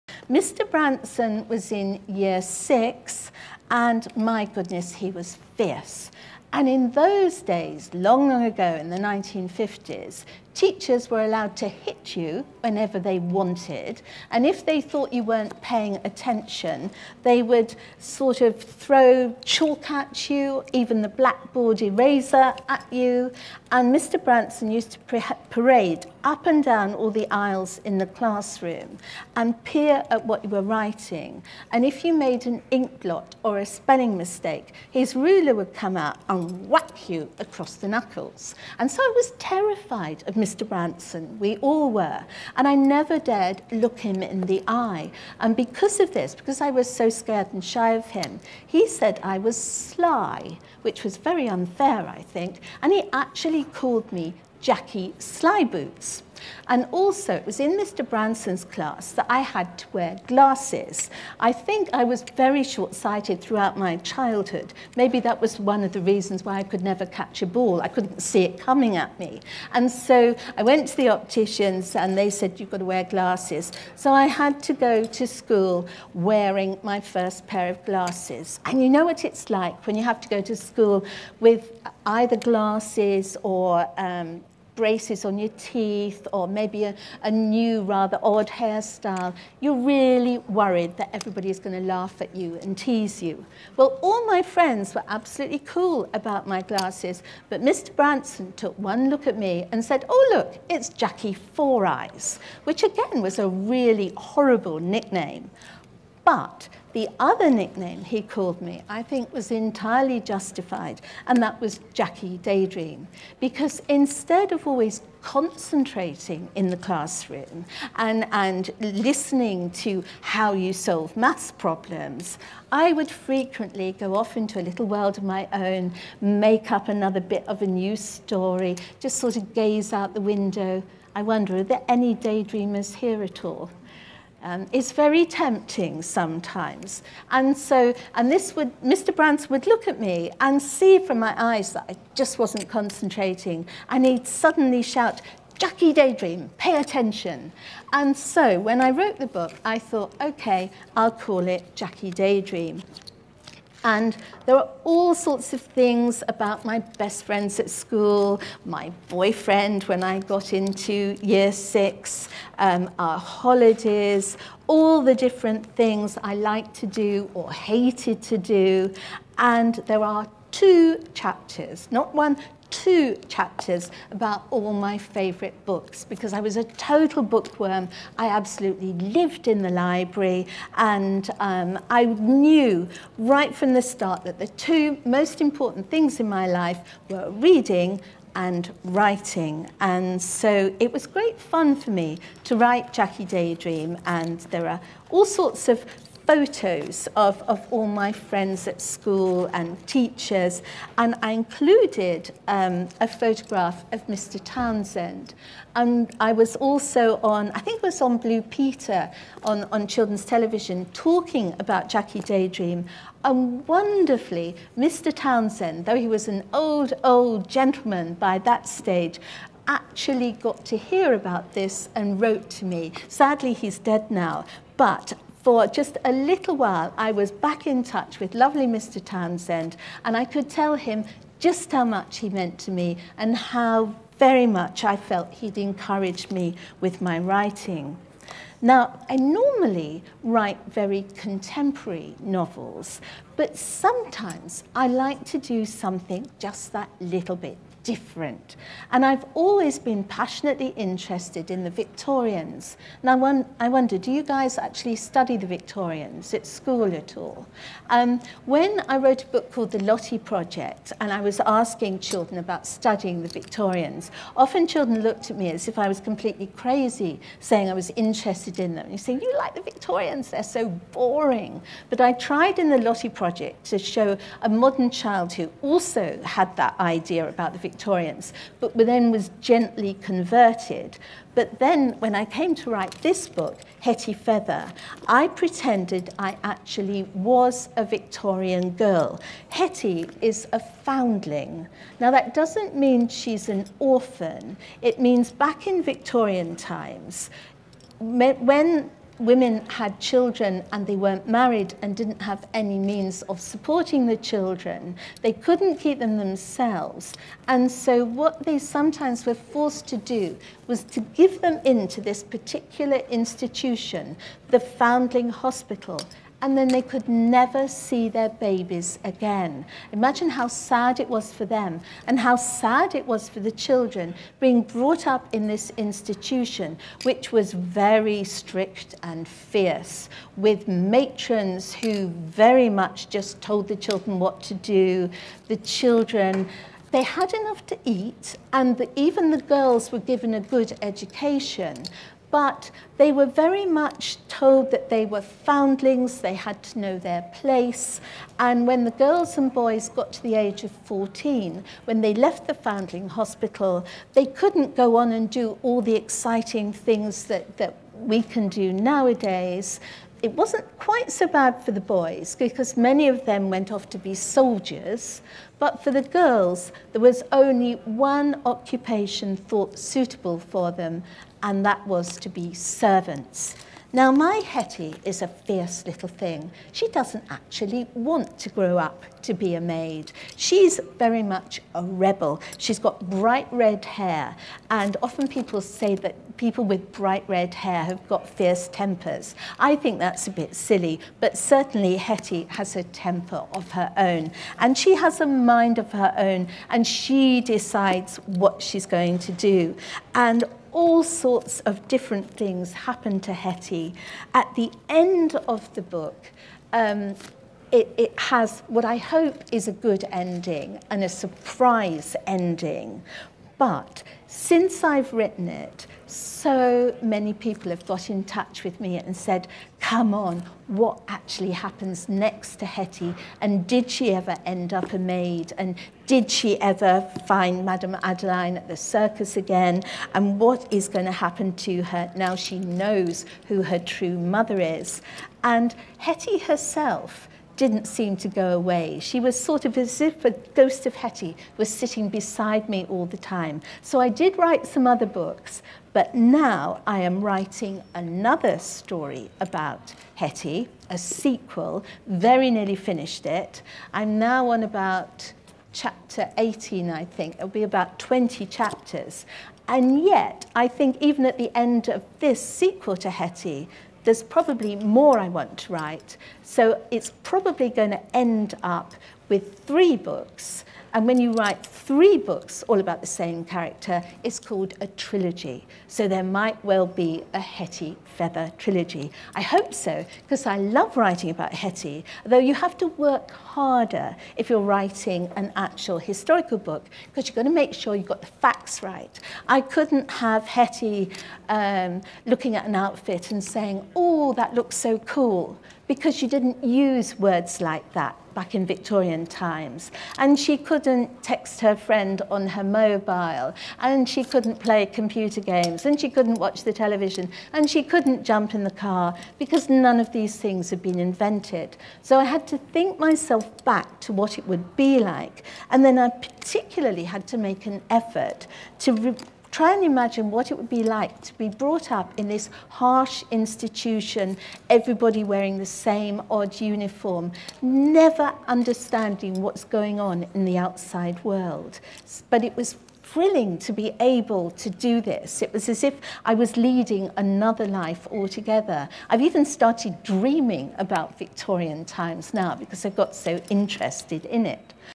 Authors Live event with Jacqueline Wilson Part 4 - Mr. Branson
This is a clip from the Authors Live event, part of the Meet Our Authors programme, with Jacqueline Wilson organised by Scottish Book Trust, which was streamed live on 27th January 2011 to 48,103 children in the UK.

In this clip Jacqueline talks about her fierce teacher Mr. Branson and how she got her nickname 'Jacky Daydream'.

For more information about the Meet Our Authors programme please